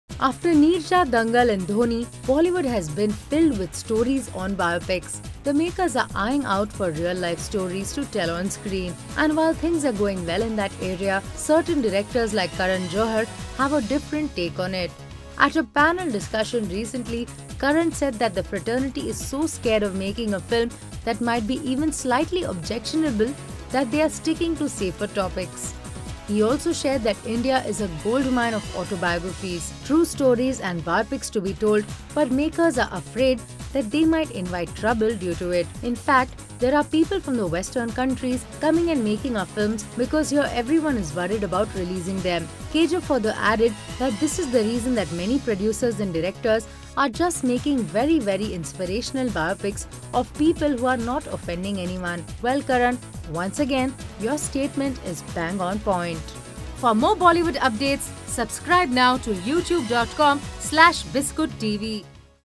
OMG! Karan Johar takes a dig on biopics in Bollywood
After Neerja, Dangal and Dhoni, Bollywood has been filled with stories on biopics. The makers are eyeing out for real life stories to tell on screen. And while things are going well in that area, certain directors like Karan Johar have a differed take on it. At a panel discussion recently, Karan Johar said that the fraternity is so scared of making a film that might be even slightly objectionable that they are sticking to safer topics. Karan Johar also shared that India is a goldmine of autobiographies, true stories, biopics to be told but makers are afraid to invite trouble due to it. In fact there people from the western countries coming and making our films because here everyone is worried about releasing them. Karan Johar further added that this is the reason that many producers and directors are just making very very inspirational biopics of people who are not offending anyone. Well Karan Johar, once again your statement is bang on point! Find out what Karan Johar has to say in this video.